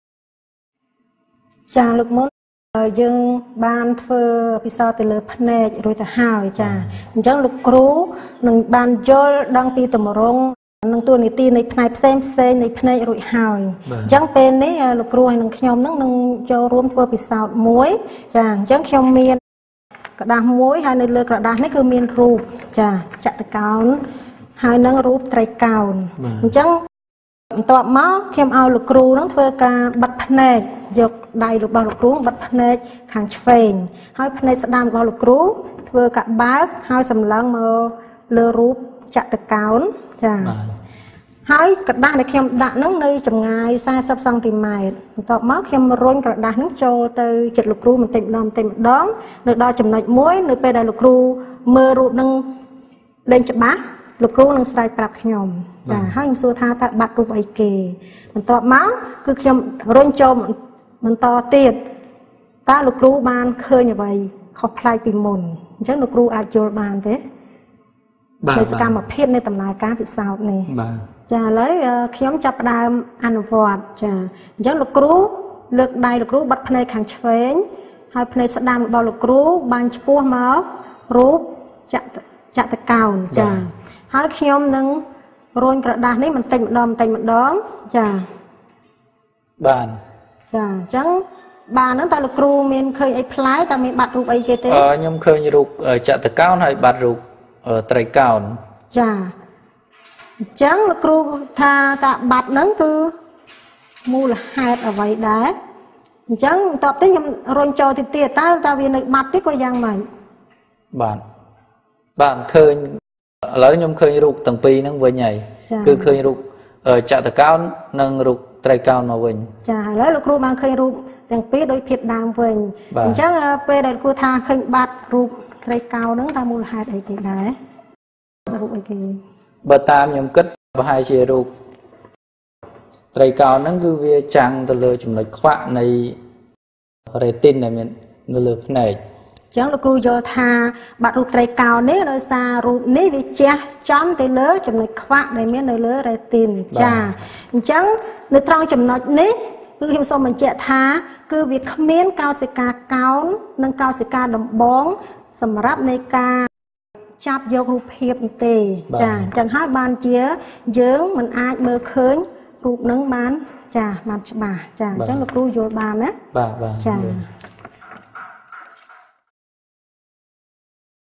វីដេអូពិសោធន៍មុខវិជ្ជាជីវវិទ្យា ផ្នែកទី៥ ៣ ចំណុចខ្វាក់លើរេទីន
គោល​បំណង​សំខាន់​នៃ​វីដេអូ​ពិសោធន៍​នេះ គឺបង្ហាញ​នូវ​វិធីពិសោធន៍​ ចំណុចខ្វាក់លើរេទីន